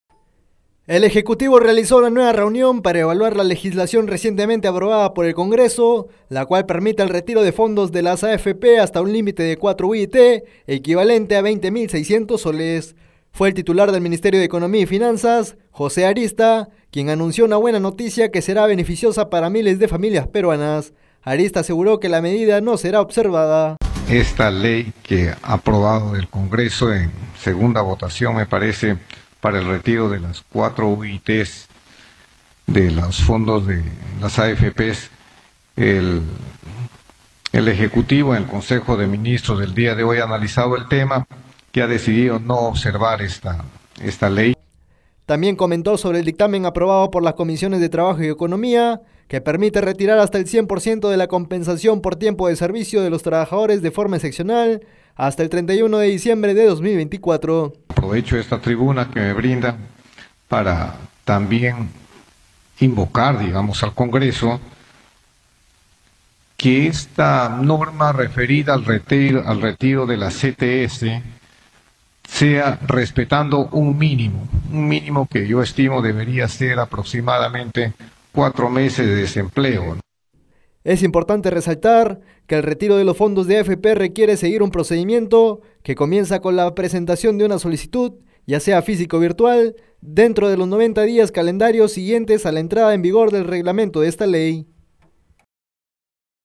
Nacional: Gobierno opta por no observar ley de retiro de fondos de AFP
El Ejecutivo realizó una nueva reunión para evaluar la legislación recientemente aprobada por el Congreso, la cual permite el retiro de fondos de las AFP hasta un límite de 4 UIT, equivalente a S/ 20 600.

Fue el titular del Ministerio de Economía y Finanzas, José Arista, quien anunció una buena noticia que será beneficiosa para miles de familias peruanas. Arista aseguró que la medida no será observada.

También comentó sobre el dictamen aprobado por las comisiones de Trabajo y Economía, que permite retirar hasta el 100% de la Compensación por Tiempo de Servicio de los trabajadores de forma excepcional hasta el 31 de diciembre de 2024.

Es importante resaltar que el retiro de los fondos de AFP requiere seguir un procedimiento, que comienza con la presentación de una solicitud, ya sea física o virtual, dentro de los noventa días calendario siguientes a la entrada en vigor del reglamento de esta ley.